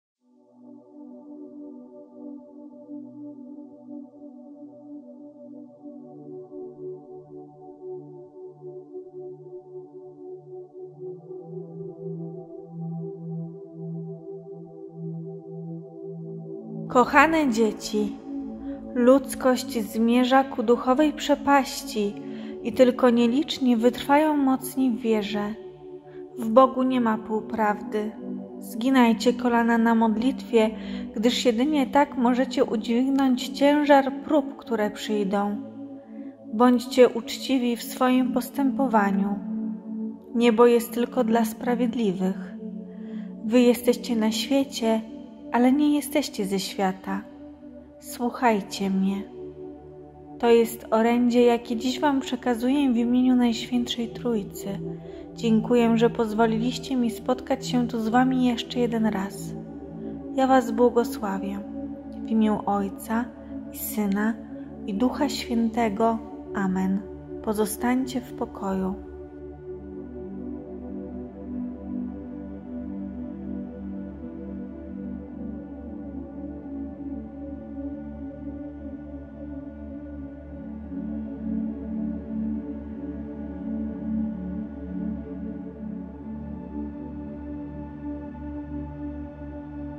🙏 Orędzie Maryi: TYLKO NIELICZNI WYTRWAJĄ | Pedro Regis (Anguera, Brazylia) 01.09.2022 🙏
Kochane dzieci, ludzkość zmierza ku duchowej przepaści, i tylko nieliczni wytrwają mocni w wierze. W Bogu nie ma półprawdy. Zginajcie kolana na modlitwie, gdyż jedynie tak możecie udźwignąć ciężar prób, które przyjdą. Bądźcie uczciwi w swoim postępowaniu. Niebo jest tylko dla sprawiedliwych. Wy jesteście na świecie, ale nie jesteście ze świata. Słuchajcie Mnie. To jest orędzie, jakie dziś wam przekazuję w Imieniu Najświętszej Trójcy. Dziękuję, że pozwoliliście Mi spotkać się tu z wami jeszcze jeden raz. Ja was błogosławię: w Imię Ojca, i Syna, i Ducha Świętego. Amen. Pozostańcie w pokoju.